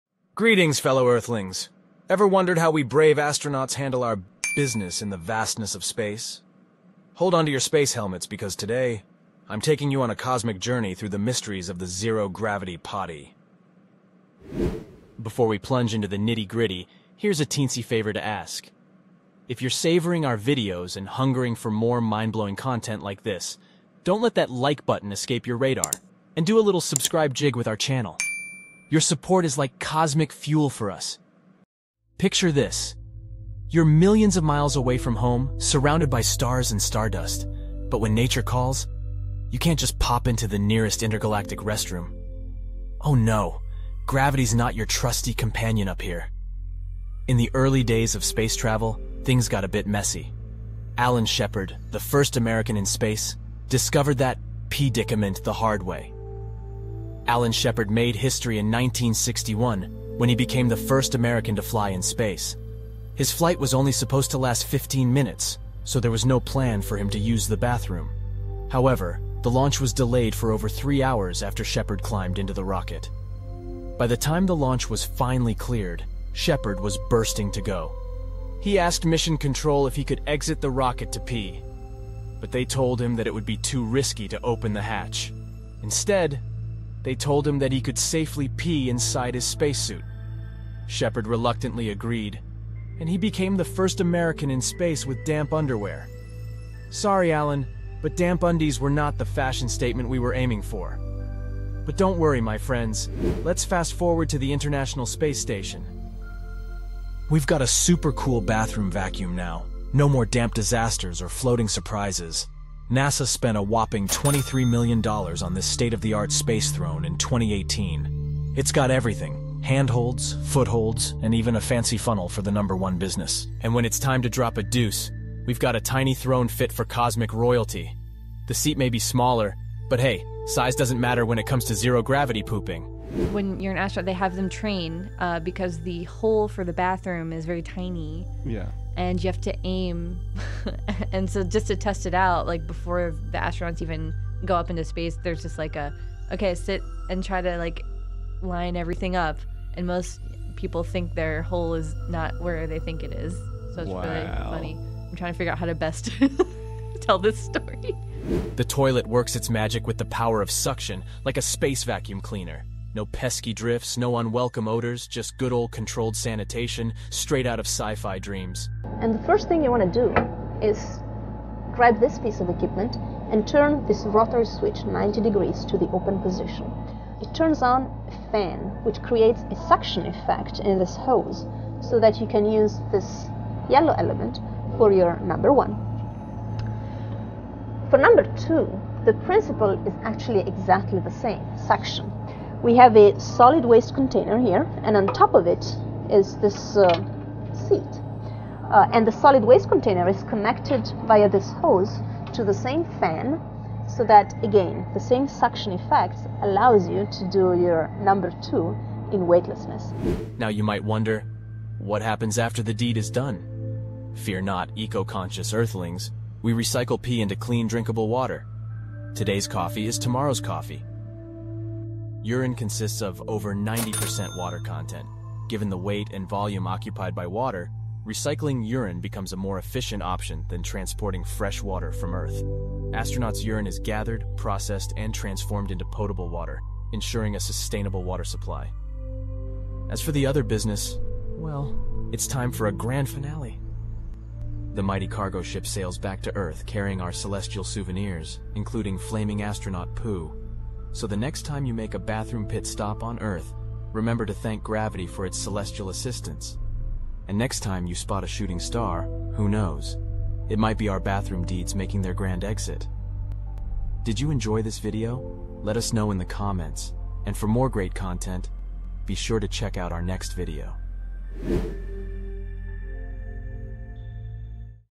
This is How You Go to Toilet in Space! 👨‍🚀🚽
🚀 Embark on an interstellar adventure like no other as we unveil the mysteries of astronaut bathrooms in outer space! 🌌 From wet britches to zero-gravity toilets, join our quirky cartoon astronaut as we explore the fascinating world of space restrooms. 🛀💫

Ever wondered how astronauts tackle the ultimate challenge of going to the bathroom in the vastness of space? Curious about the evolution of cosmic commodes? Prepare to be amused and amazed as we take you through the comical mishaps of early space missions and the ingenious solutions NASA has devised over the years.

Discover the high-tech diapers worn during spacewalks, the magic of vacuum tubes for urine, and the all-new Universal Waste Management System designed to accommodate the unique needs of both male and female astronauts. We'll even spill the beans (or should we say turds?) on how floating waste is managed in the depths of space.

Join our cosmic journey as we sprinkle in a dash of humor, a pinch of science, and a whole lot of fun! Don't miss out on this gravity-defying episode that's bound to leave you both enlightened and chuckling among the stars. 🌟🪐

Chapters:
00:00 - Intro
00:47 - Story of Alan Shepard
01:43 - 23 Million Dollar Space Toilet
02:38 - How Does It Work?
03:30 - Turns Urine into Drinking Water
04:00 - Poop burns up like 'Shooting Stars'!
04:23 - Outro